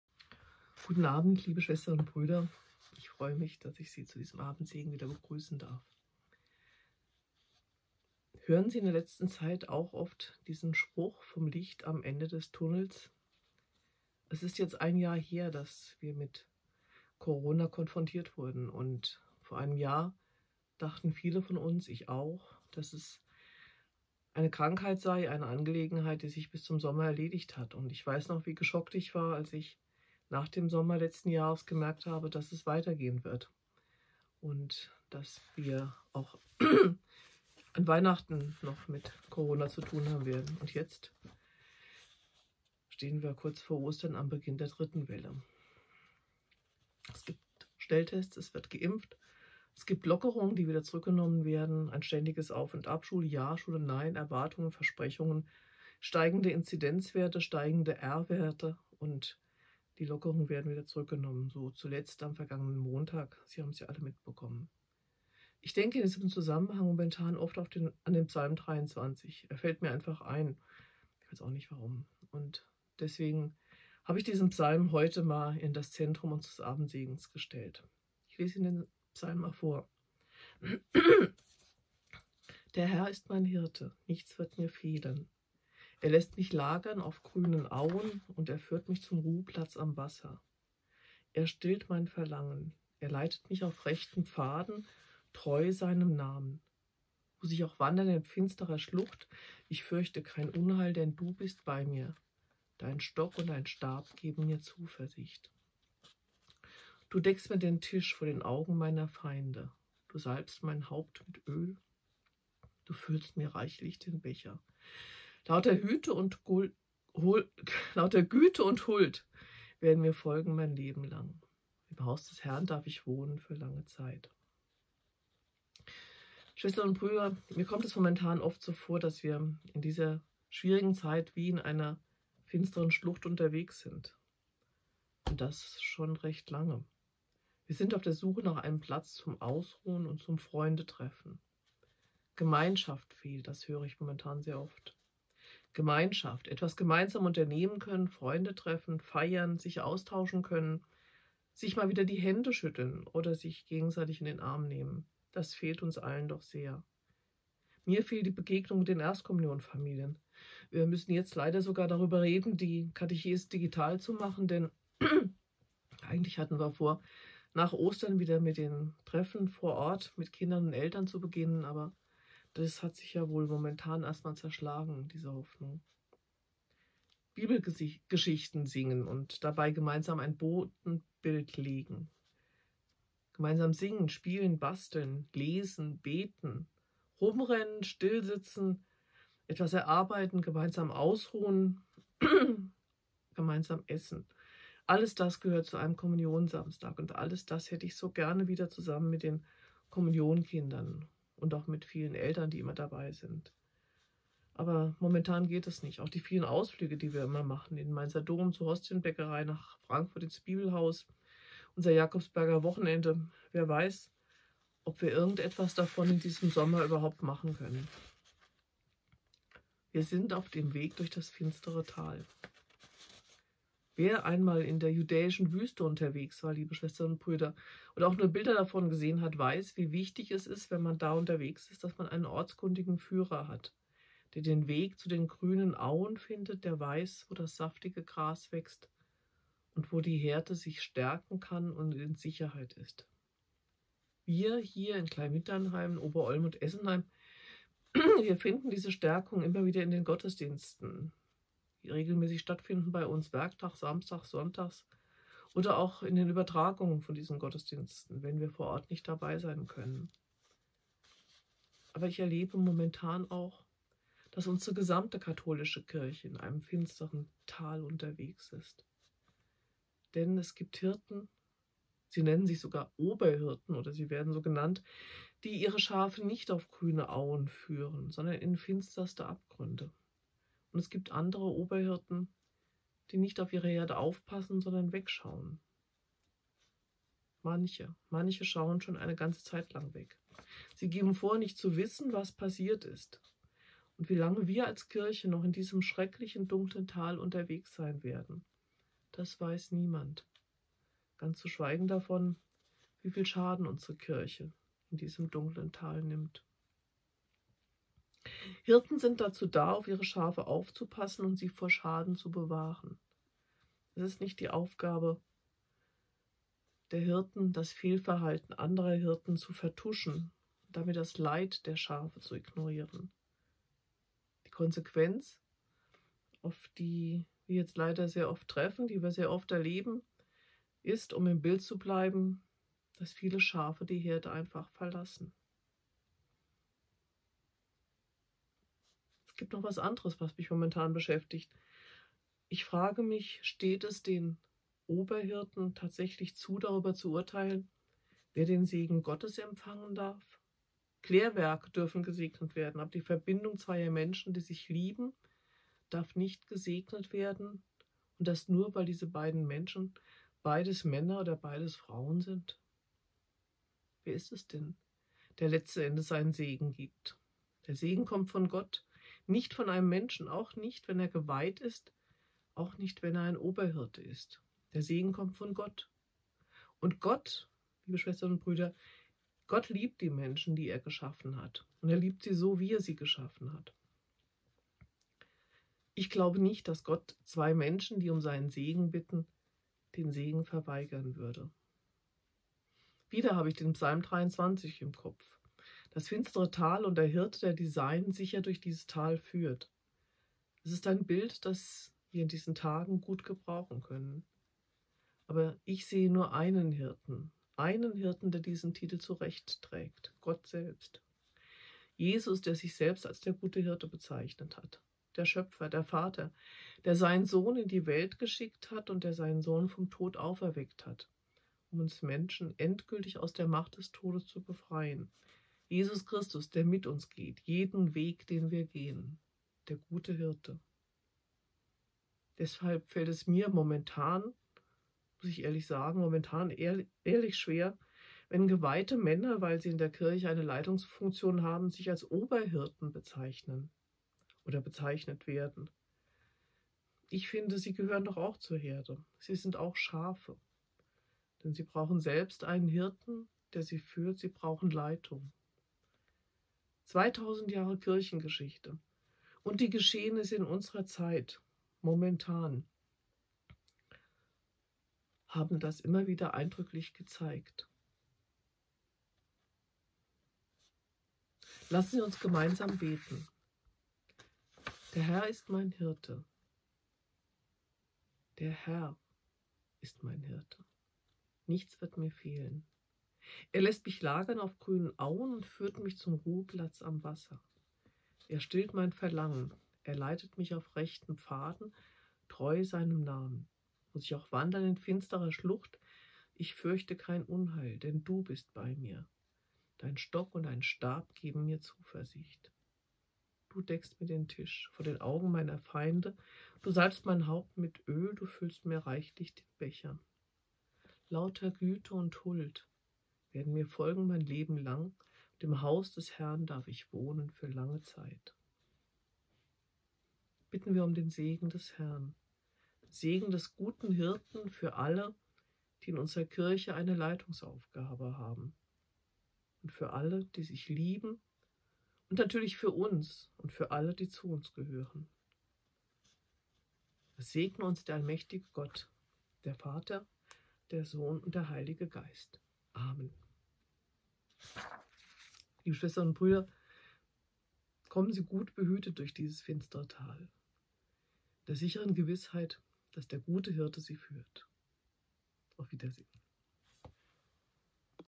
Abendsegen am 24.03.2021 von Frau Krämer-Kost | Ober-Olm
Frau Krämer-Kost mit ihrem Abendsegen am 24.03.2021.